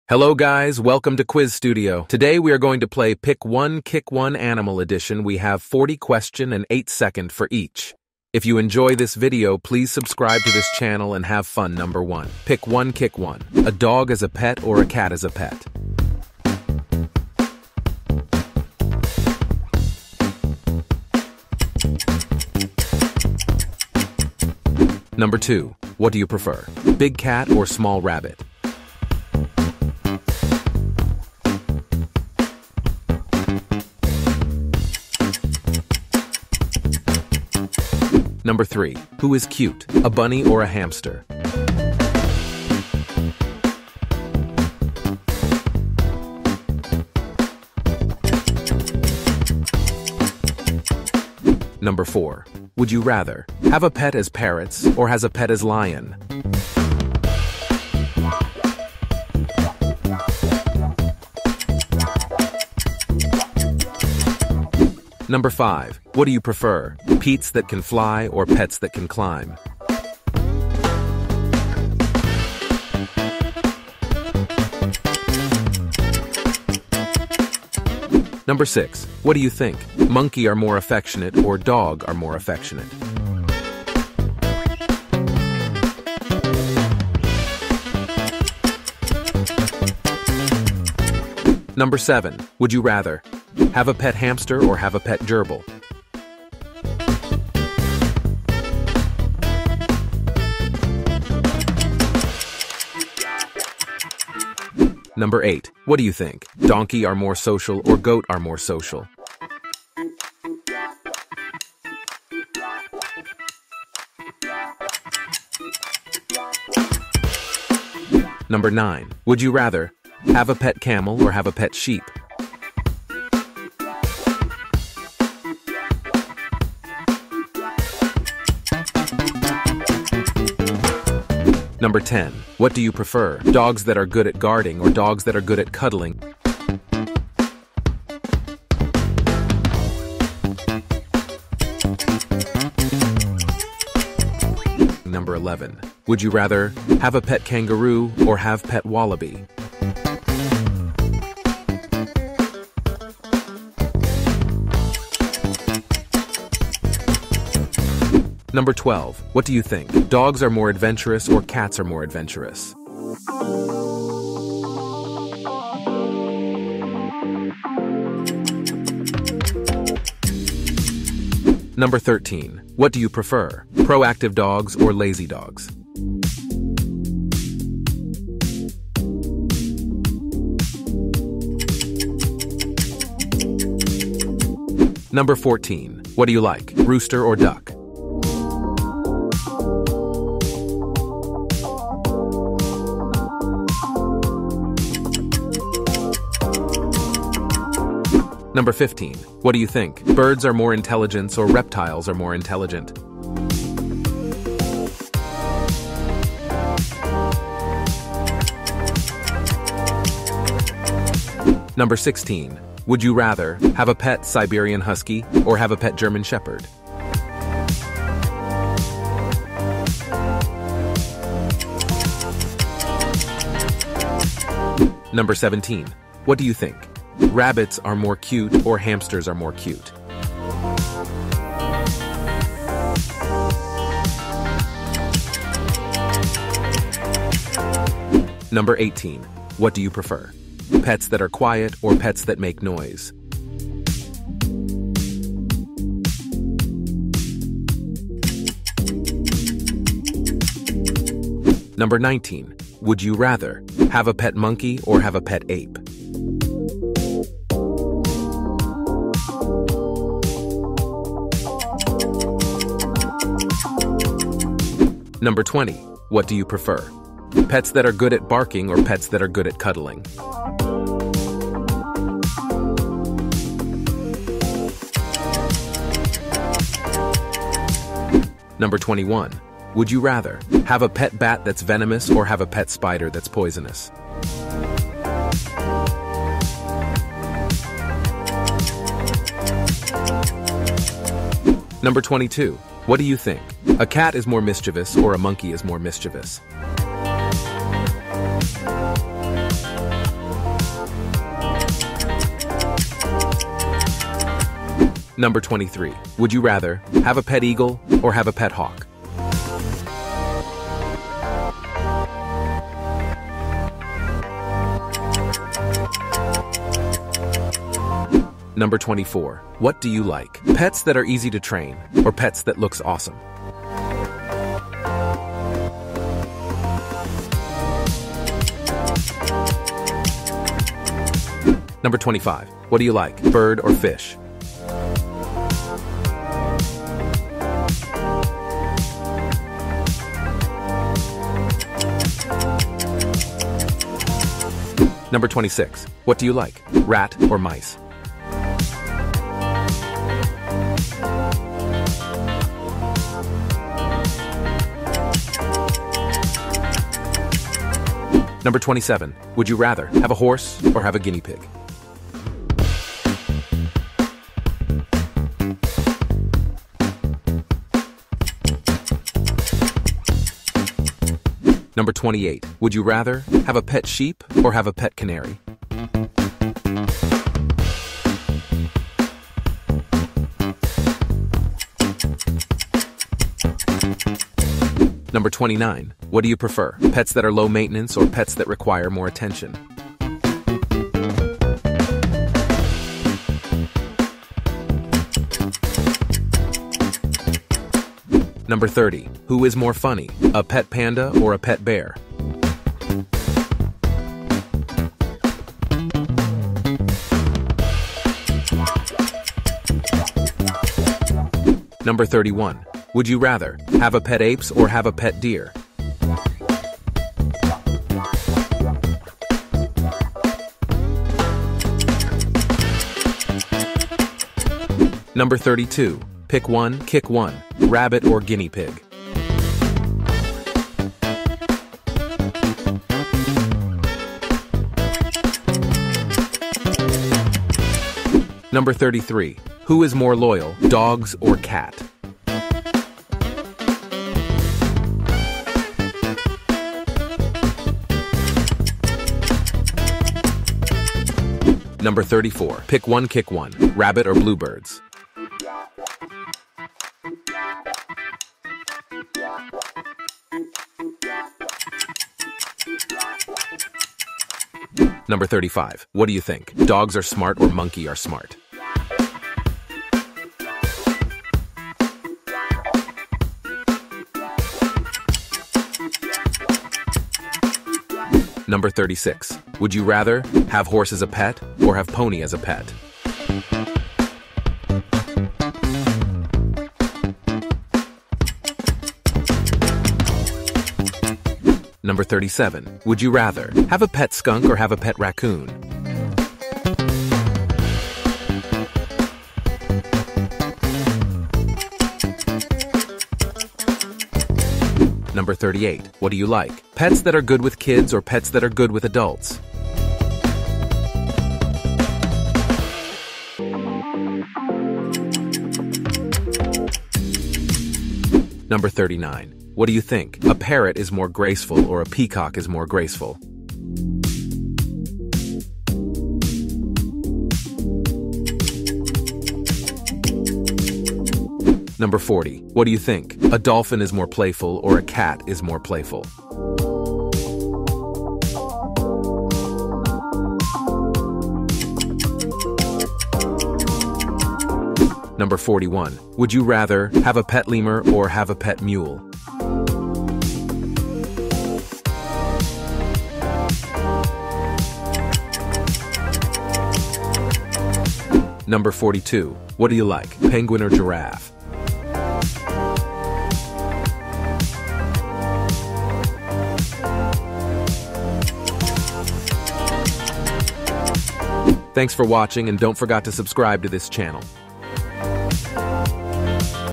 Pick One Kick One - Animals Edition 🐵🐶🐘
"Pick One Kick One - Animal Edition" is an exciting and interactive game that challenges players to make difficult choices between two different animals. It combines the thrill of decision-making with the charm of the animal kingdom, making it an engaging experience for all ages.

The game presents players with pairs of animals, carefully selected to evoke a sense of dilemma. Each animal comes with its unique traits, strengths, and adorable characteristics. Players are asked to "pick one" animal they prefer from the pair and "kick one" animal they prefer less, symbolizing a playful and harmless elimination process.

As players progress through the game, they encounter a wide range of animals from various habitats and ecosystems. From majestic creatures like lions and tigers to the adorable and tiny like kittens and puppies, the choices become increasingly challenging. The game also features lesser-known species, giving players the opportunity to learn and discover fascinating facts about wildlife while having fun.

The objective of "Pick One Kick One - Animal Edition" is not just to pick favorites, but to encourage appreciation for the diverse beauty of the animal kingdom. It highlights the importance of understanding and protecting all creatures, no matter how big or small, common or rare they may be.

The game can be enjoyed solo, allowing players to contemplate their decisions thoughtfully, or in a group setting, sparking lively discussions and friendly debates over which animal to pick or kick. The stunning visuals and delightful animations bring the animals to life, creating an immersive experience that draws players deeper into the world of wildlife.